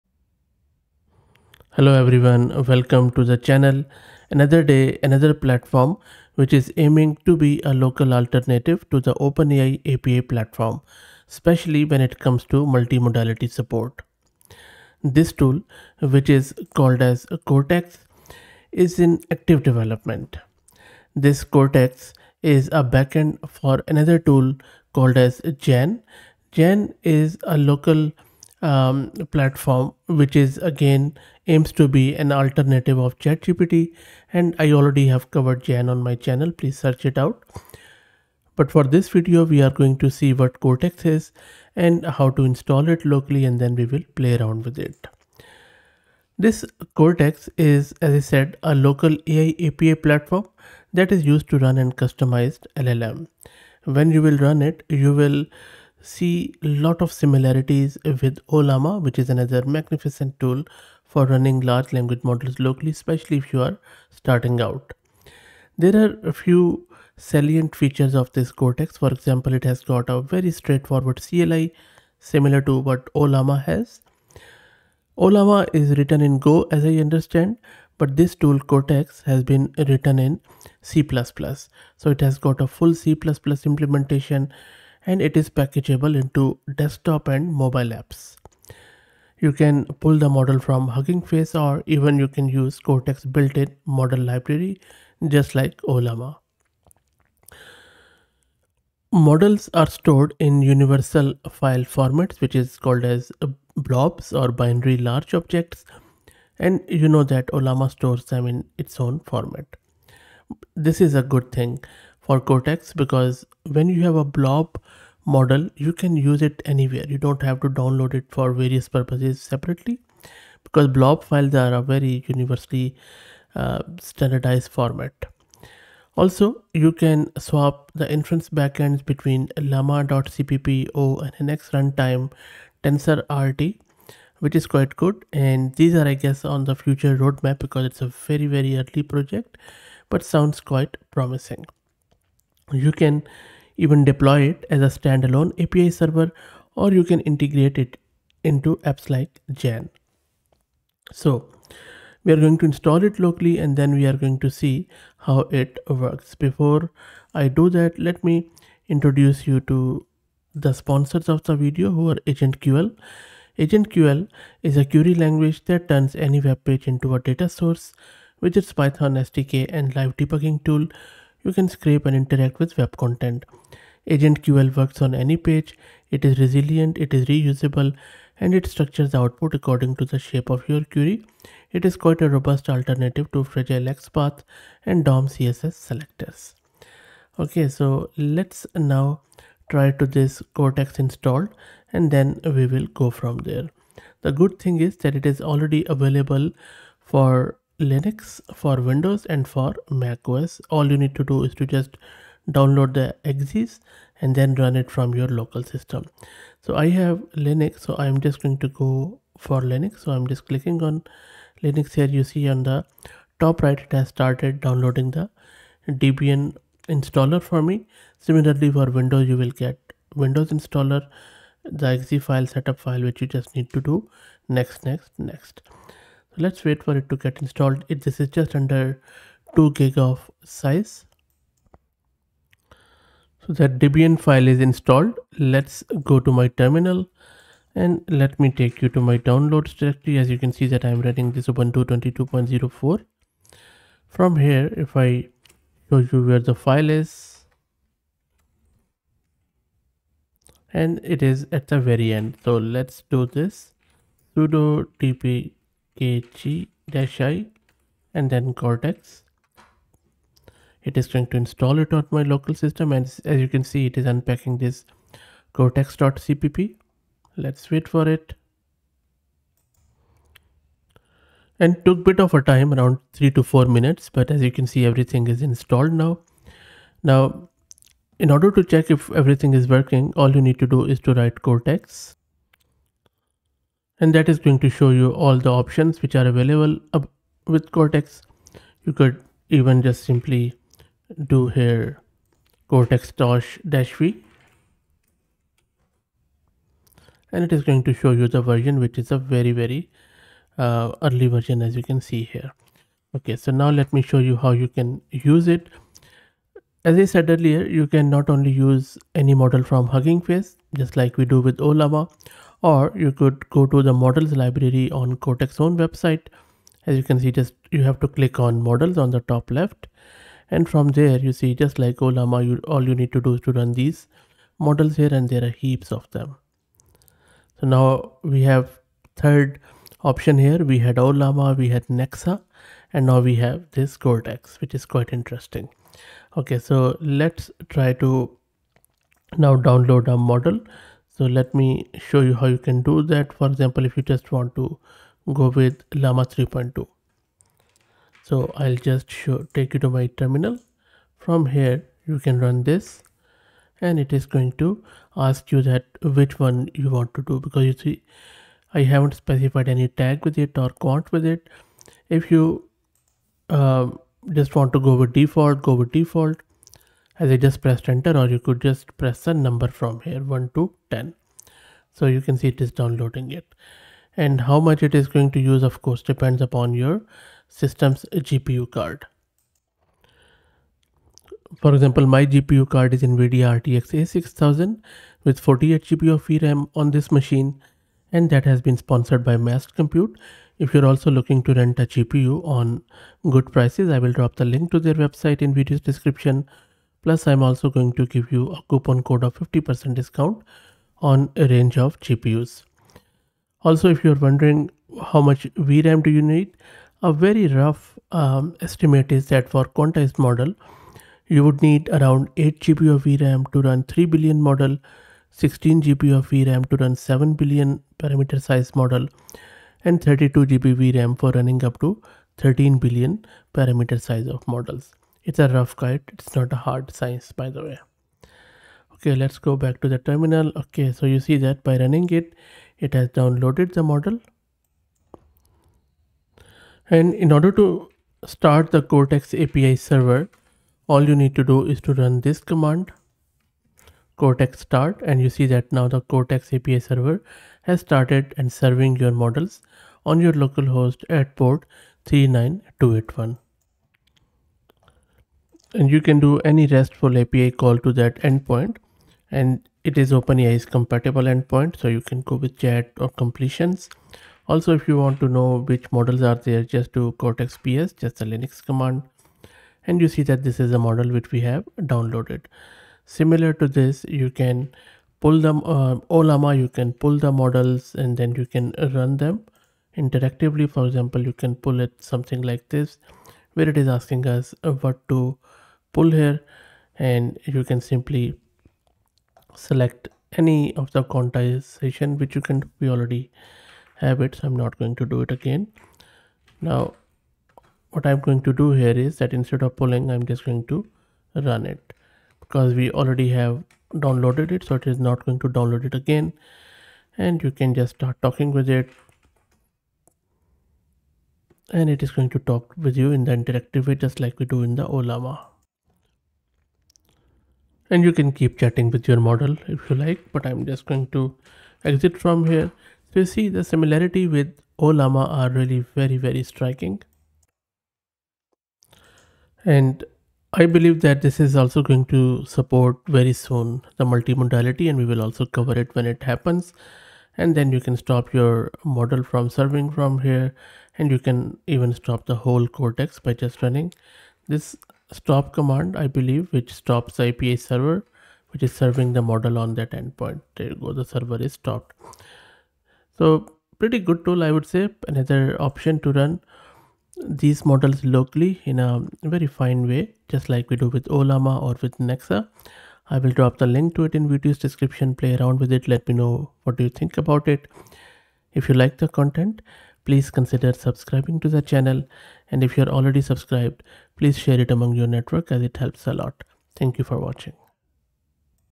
Cortex Local AI API Platform - A Local Alternative to Nexa and Ollama
This video shows how to install Cortex.cpp which is a Local AI API Platform that is used to run and customize LLMs.